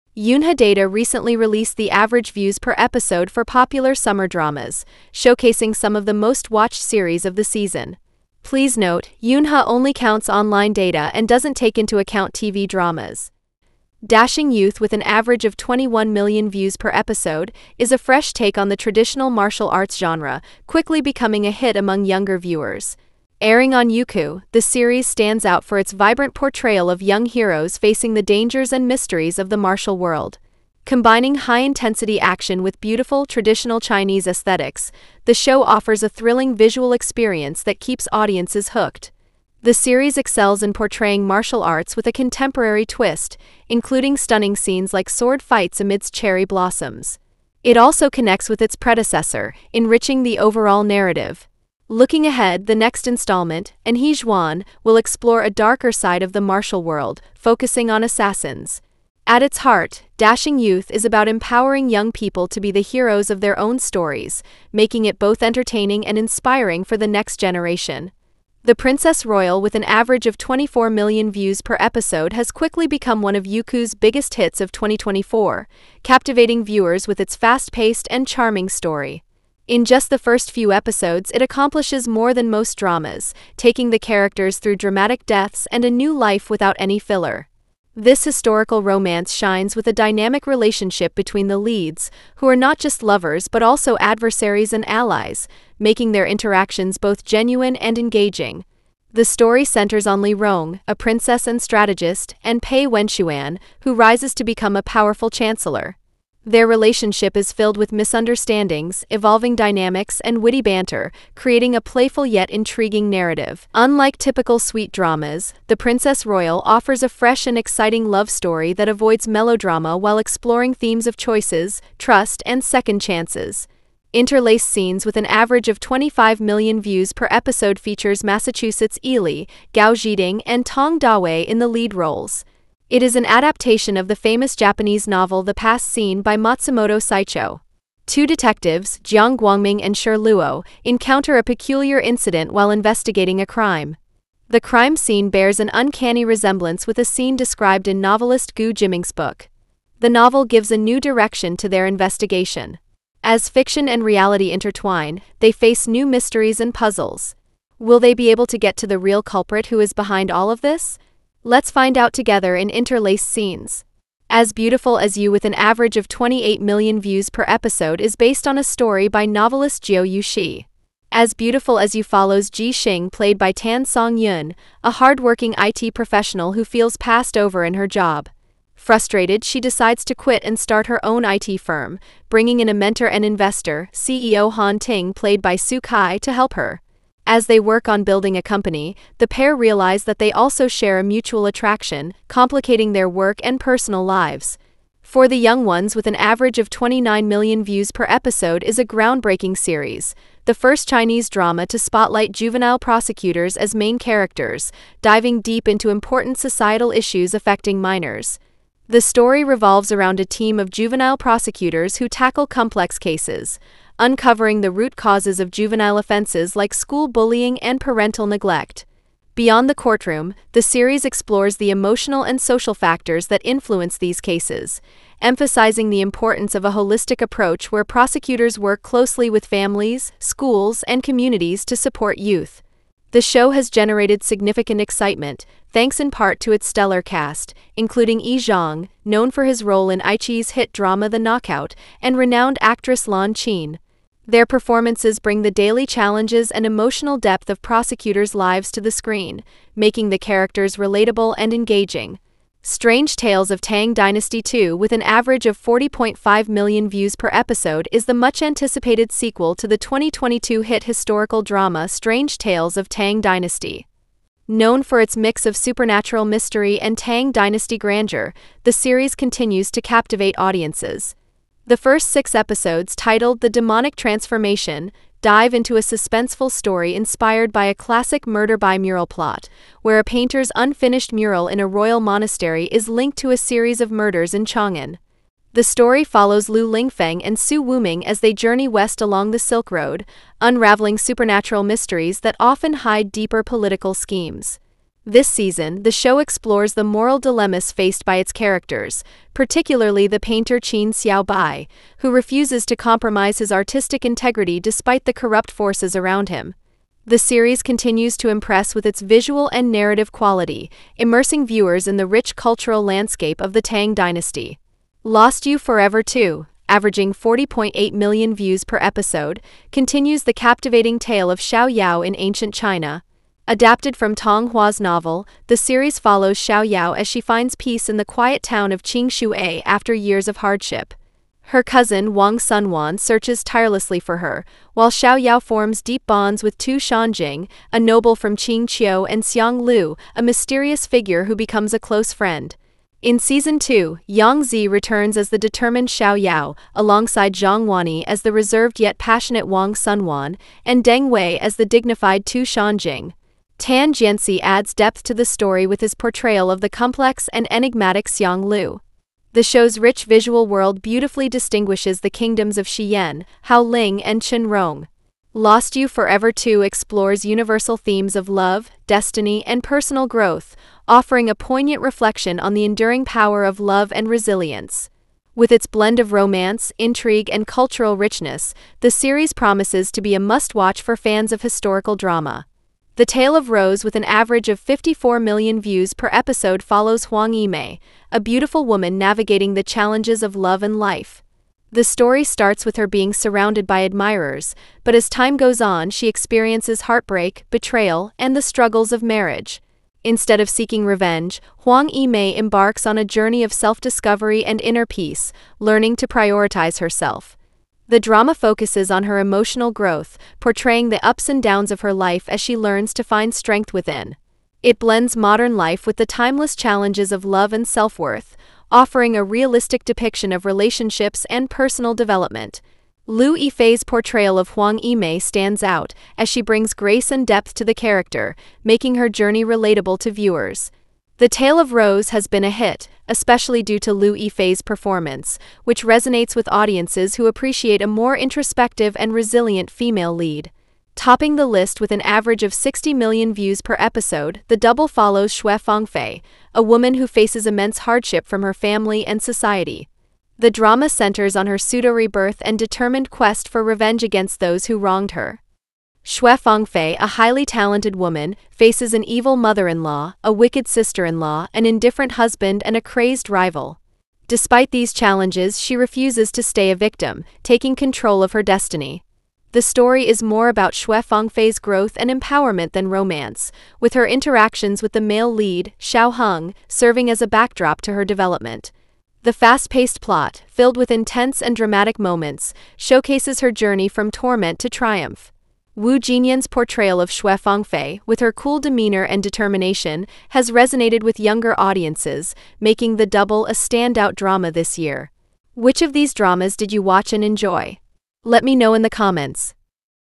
Top 10 Most Viewed Chinese Summer Dramas Of 2024 (Number 1 Will SURPRISE You)
Yunhe Data recently released the average views per episode for popular summer dramas, showcasing some of the most-watched series of the season. Yunhe only counts online data and doesn’t take into account tv dramas.